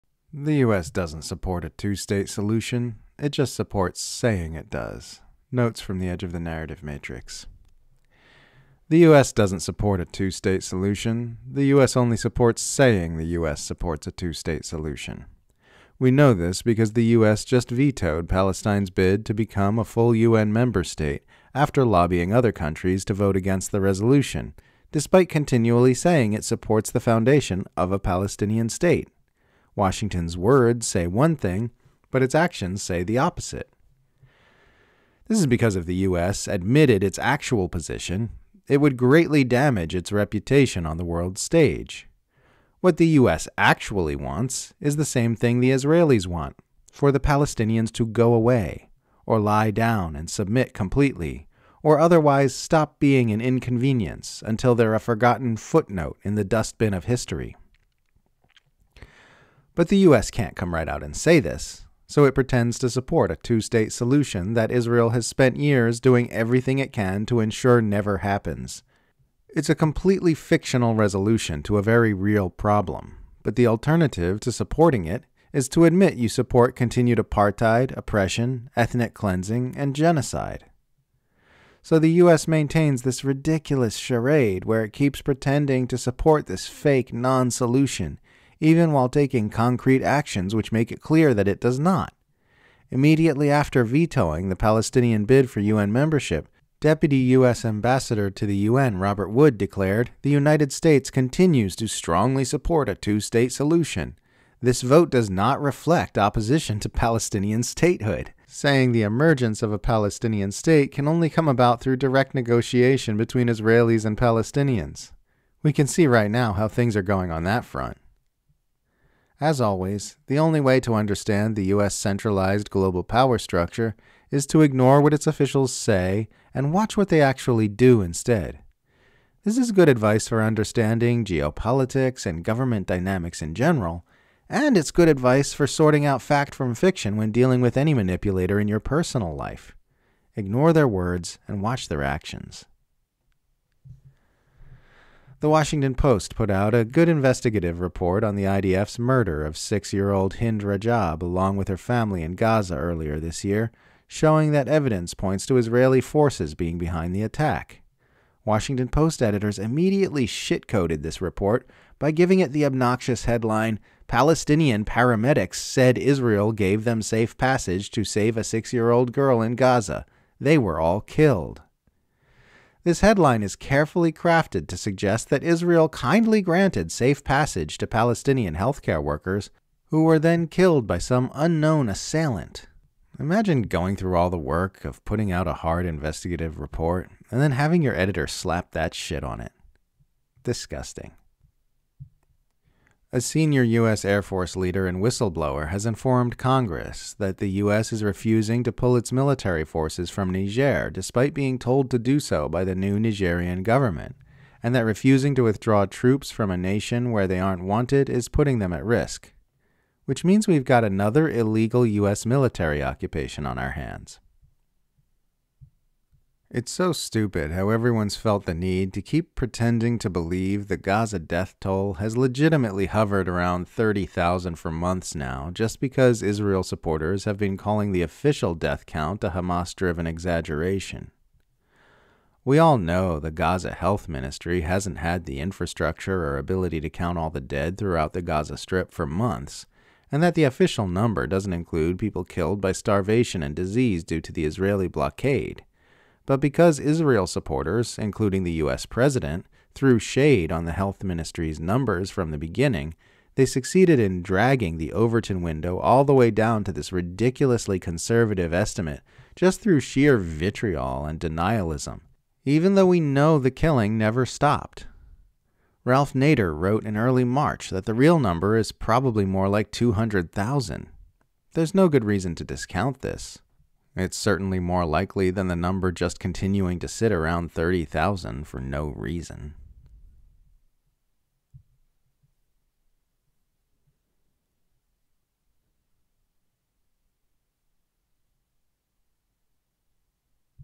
The US Doesn’t Support A Two-State Solution, It Just Supports Saying It Does
The US doesn't support a two-state solution, the US only supports saying the US supports a two-state solution. We know this because the US just vetoed Palestine's bid to become a full UN member state, after lobbying other countries to vote against the resolution - despite continually saying it supports the foundation of a Palestinian state. Washington's words say one thing, but its actions say the opposite.